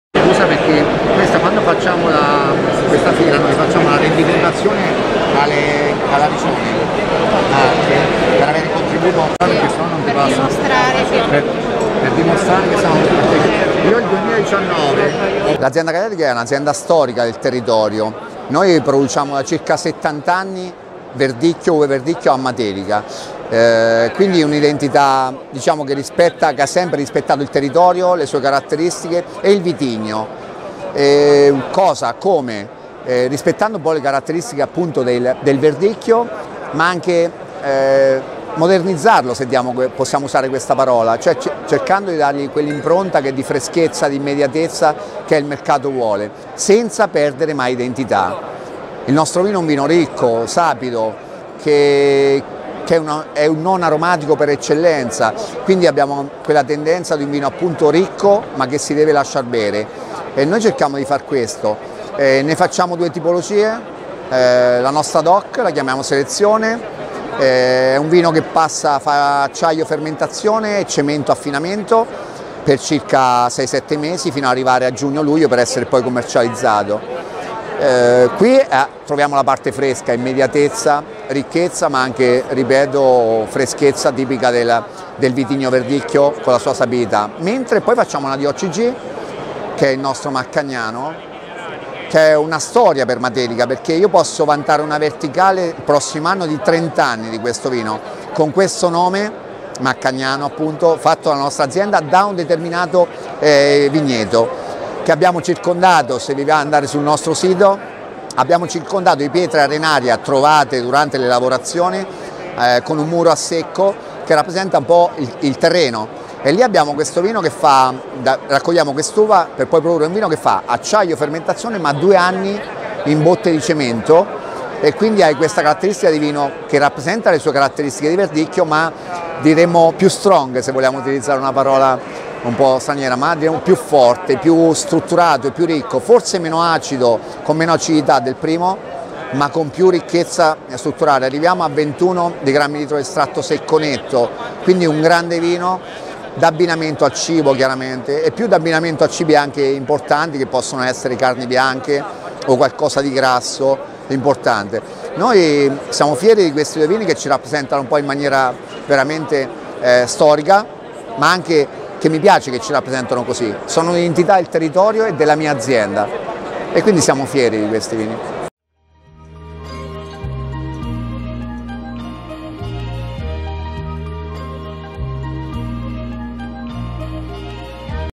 Gagliardi, oltre 60 anni di Verdicchio di Matelica
Una cantina che vanta tutti i primati possibili nel proprio territorio, avendo creduto prima e più degli altri nelle virtù del vino identitario di questa vocata area enoica marchigiana, merito di lavoro appassionato e competente: ce ne parla Umberto Gagliardi